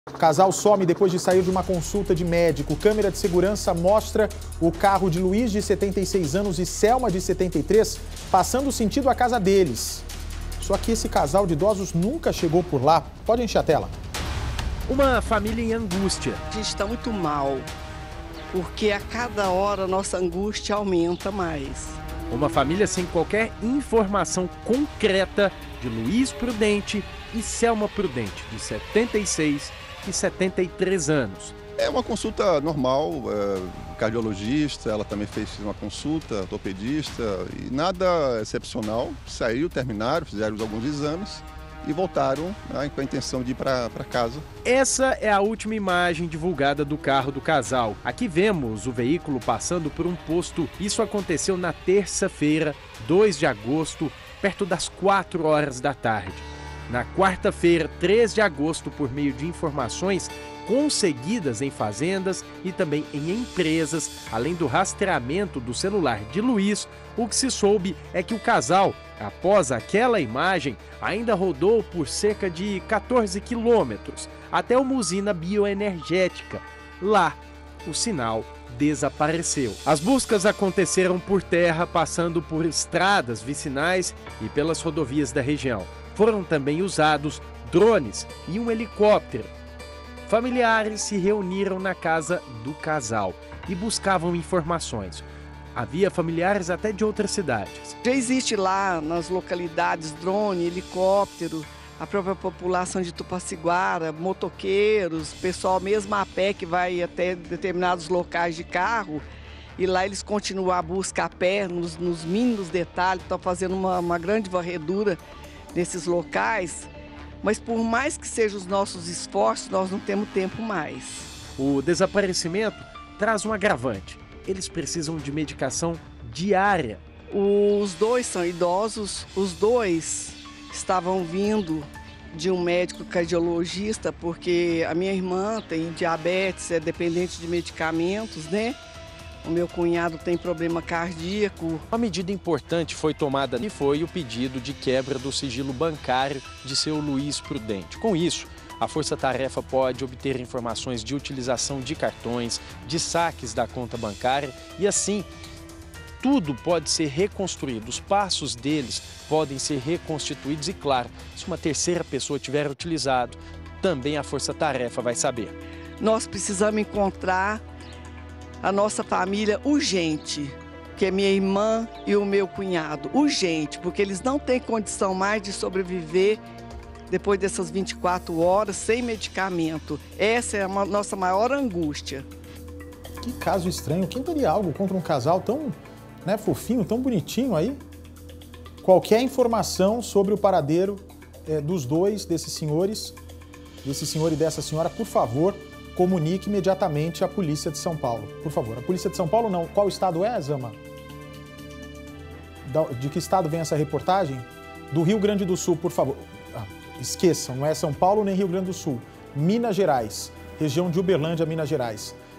Casal sai de consulta médica e desaparece em Minas Gerais
Um casal sumiu após sair de uma consulta médica em Uberlândia (MG). Os celulares dos dois foram rastreados e o último sinal mostrou que o casal estava perto de uma usina. Buscas foram feitas no local, mas Luiz, de 76 anos, e Selma, de 73 anos, não foram encontrados. Imagens de câmera de segurança de um posto de gasolina mostram o carro da família passando em sentido à casa deles, mas eles nunca chegaram.